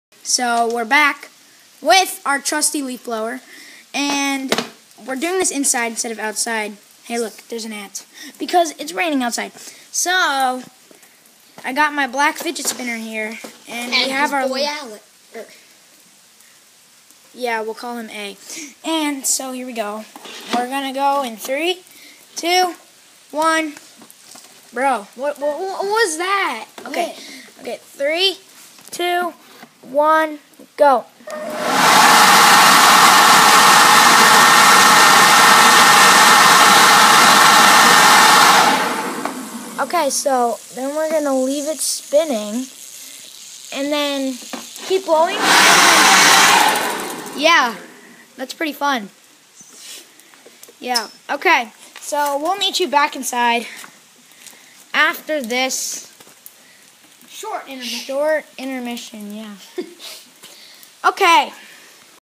Spinning a Fidget Spinner with a leaf blower as fast as we can
There are other tools but we only use the leaf blower.